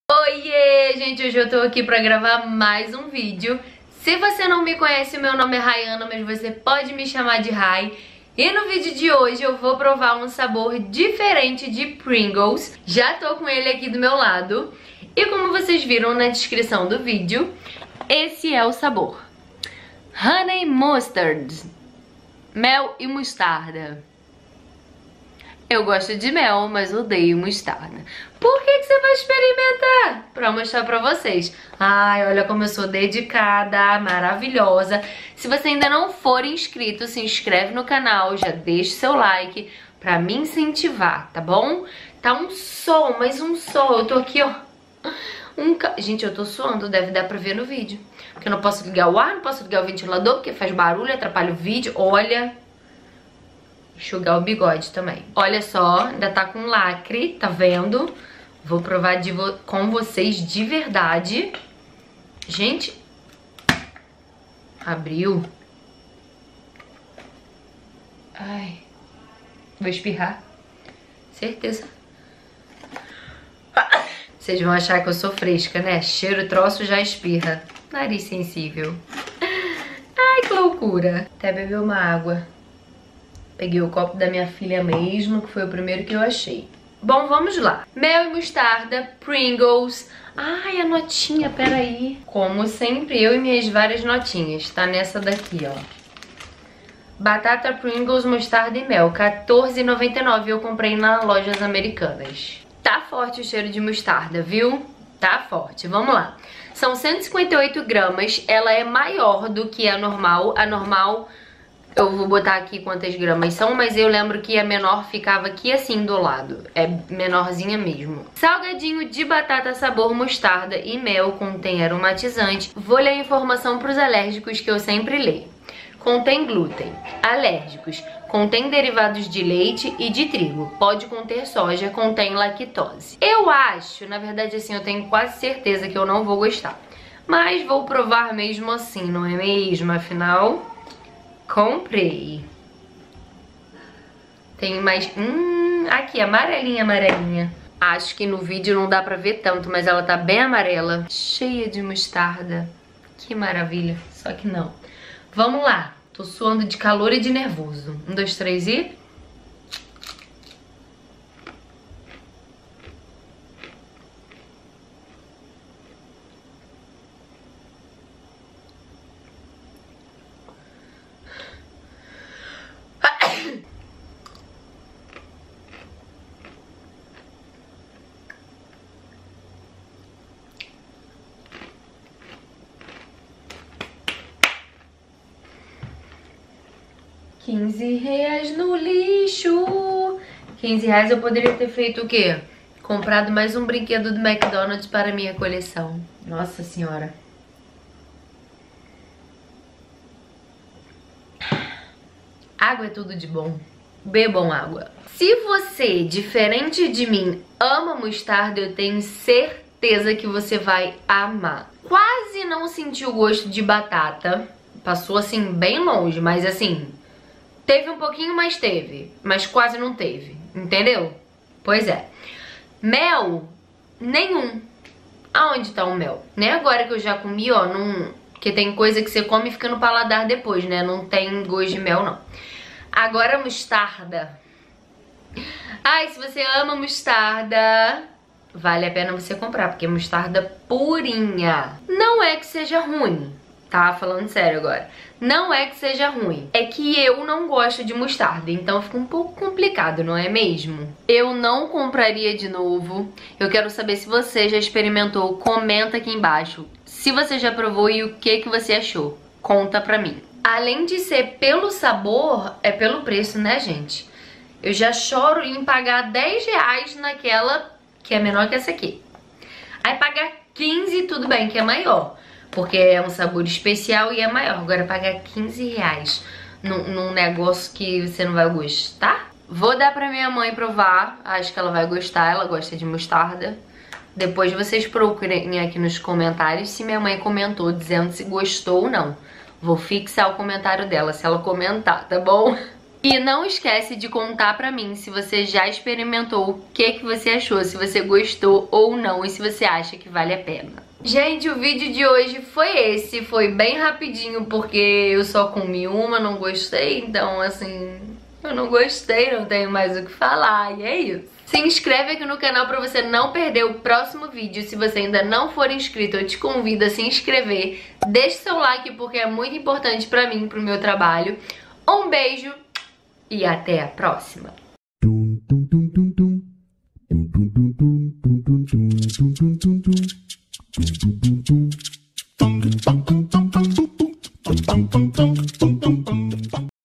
PRINGLES DE MOSTARDA E MEL - NOVO SABOR DE BATATA PRINGLES
Oieee, no vídeo de hoje eu experimentei a Pringles de mel e mostarda.

Quer conversar comigo?